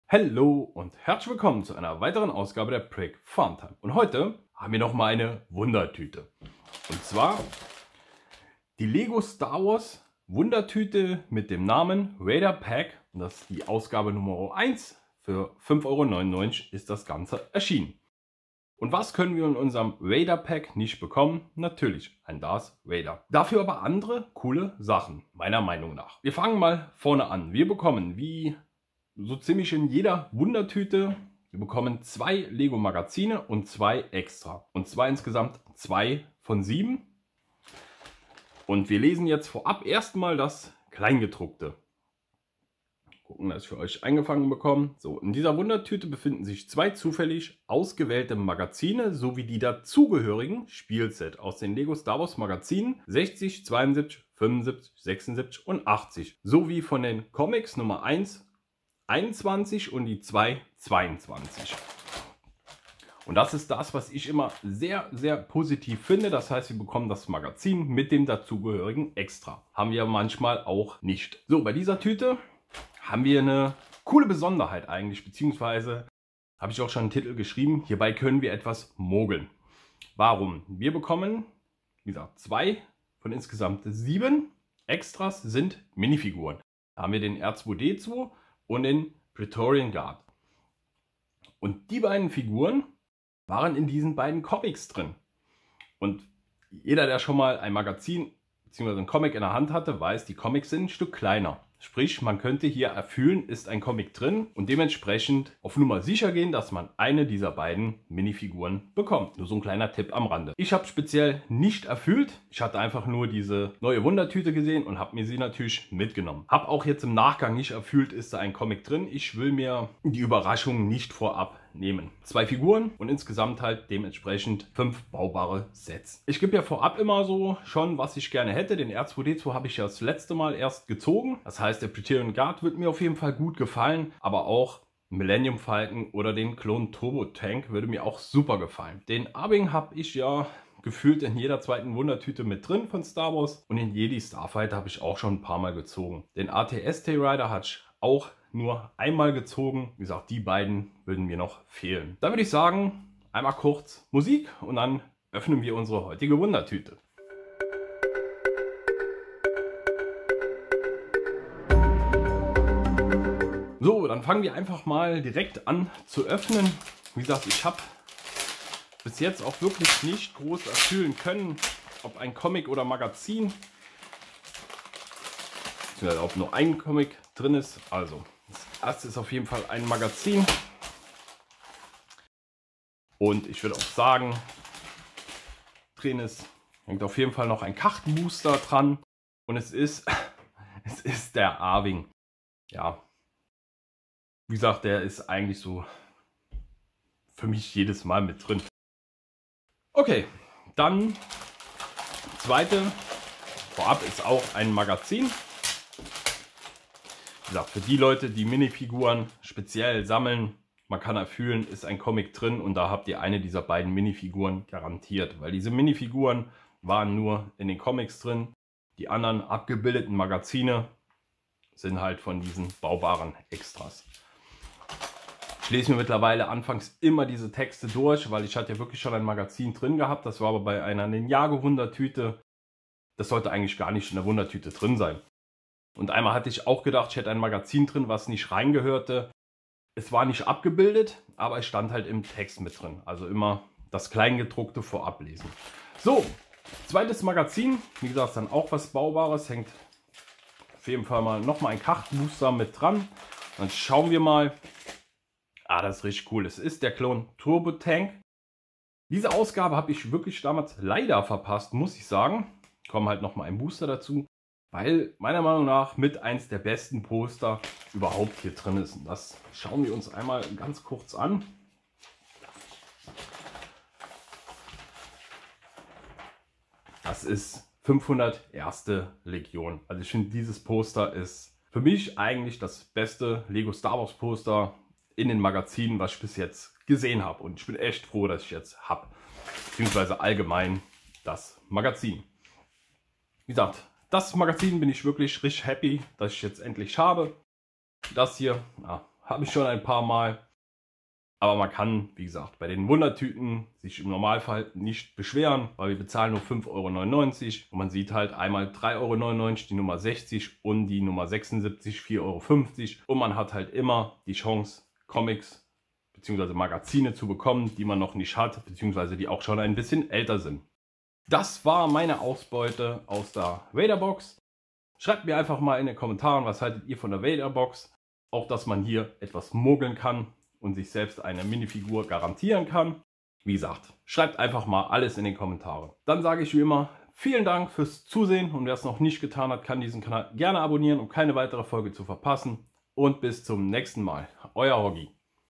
Vader-pack ohne Vader aber mit Mogel Möglichkeit😁😁! LEGO STAR WARS Wundertüte Vader-pack NR.1
Hello und Herzlich Willkommen zu einer Weiteren Ausgabe der Brick Fun Time.
Heute schauen wir uns mal die *LEGO STAR WARS Wundertüte VADER-Pack Nr.1* etwas genauer an .
Auch in dieser Ausgabe bekommen wir 2 Magazine mit den Dazugehörigen LEGO Extras.

Ich bin Gespannt und Los gehts.

Lego® und Lego®- Logos sind Markenrechtlich geschützt. Die Nutzung dient lediglich der eindeutigen Identifikation der Produkte und soll keine Verletzung der Schutzrechte darstellen.